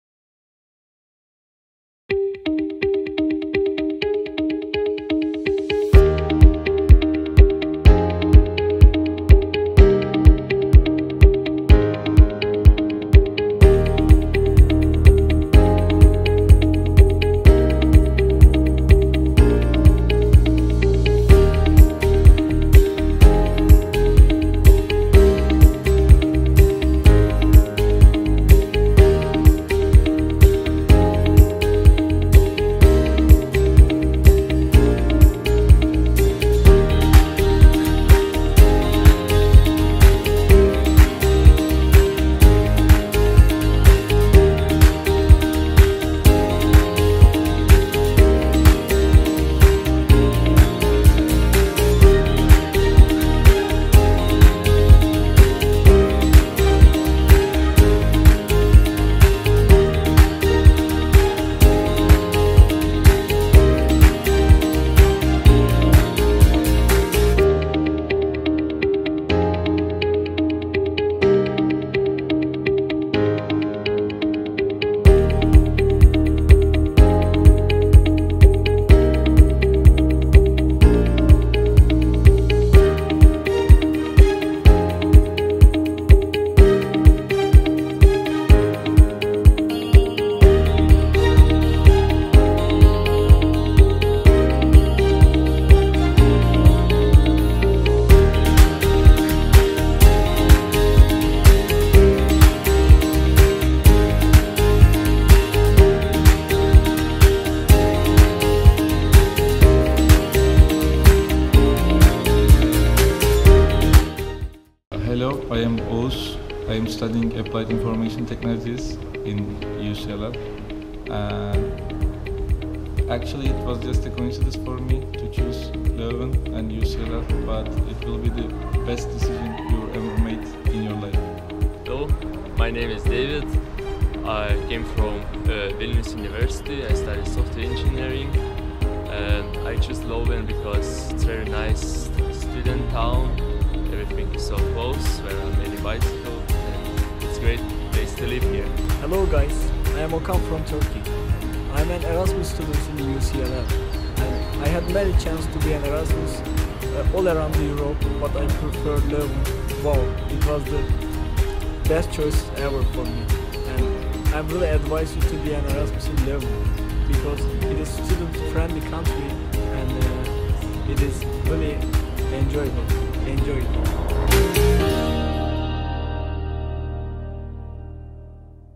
Impressions by exchange students from the spring semester 2016 at UC Leuven-Limburg
IT exchange students from Istanbul (Turkey) and Vilnius (Lithuania) share their impressions of the Spring semester 2016 at UC Leuven-Limburg.